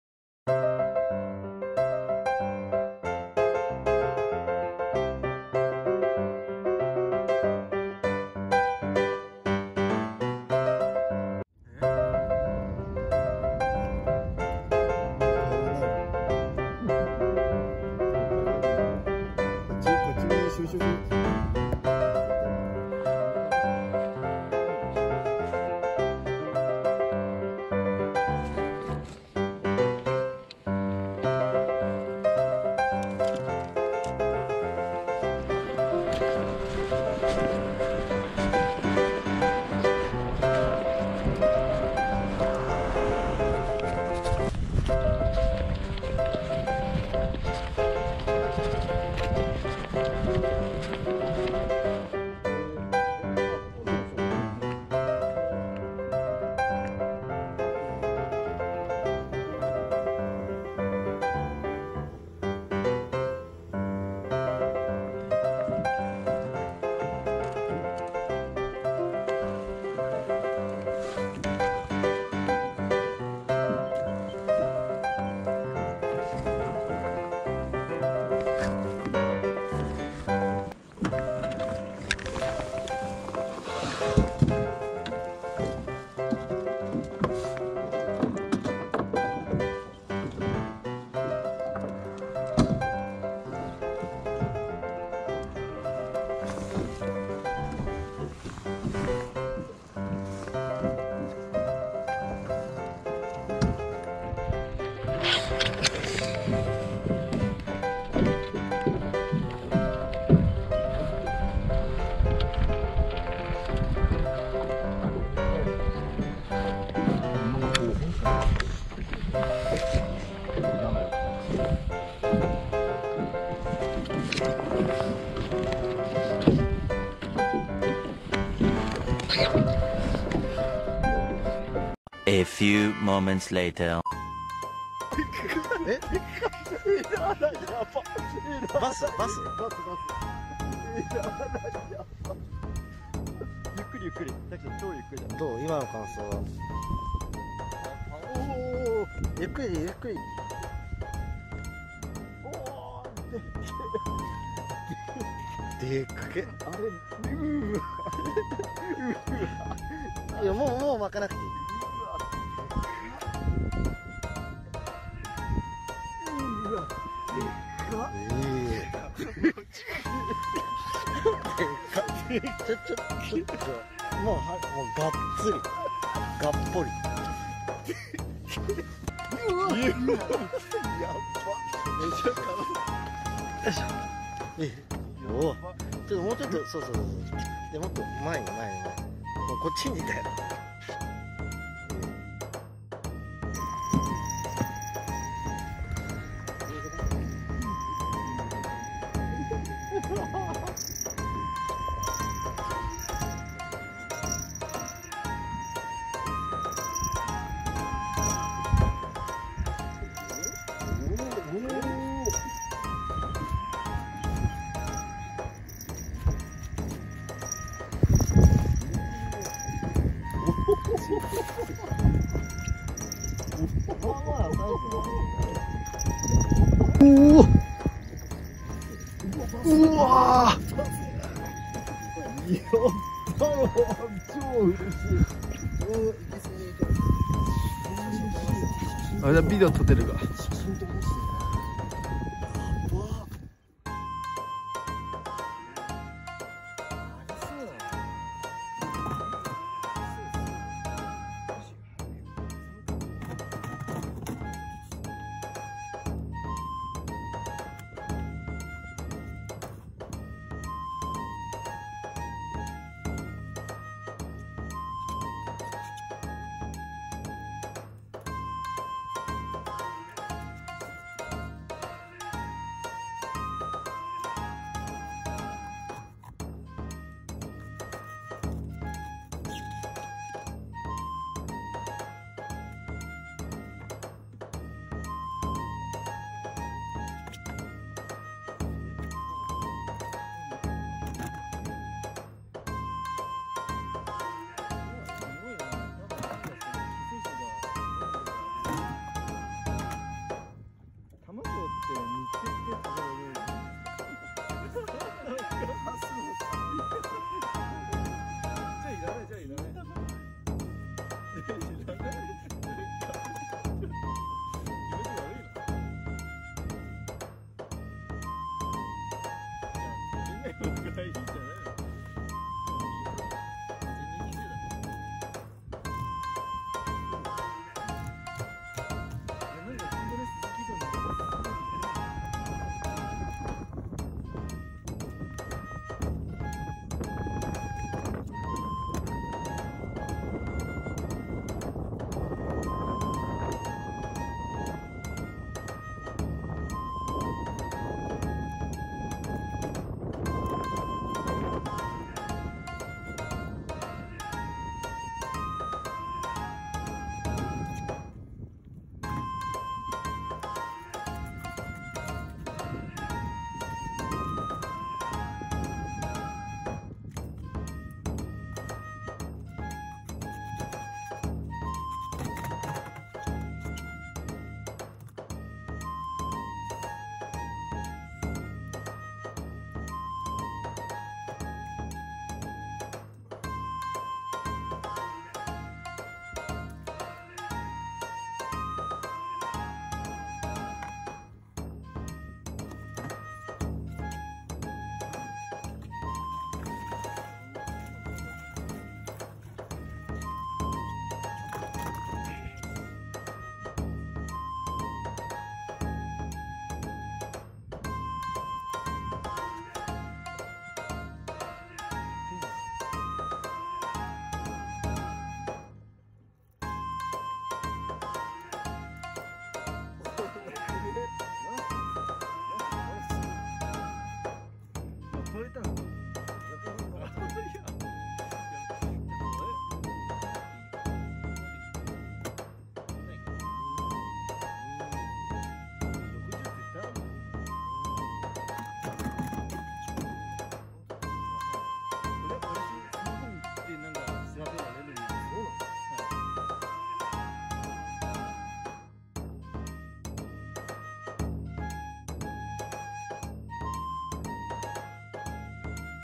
箱根芦ノ湖爆钓大嘴黑鲈黑鲈！！
大鱼吃小鱼小鱼吃虾米！日本神奈川箱根芦之湖钓大嘴黑鲈！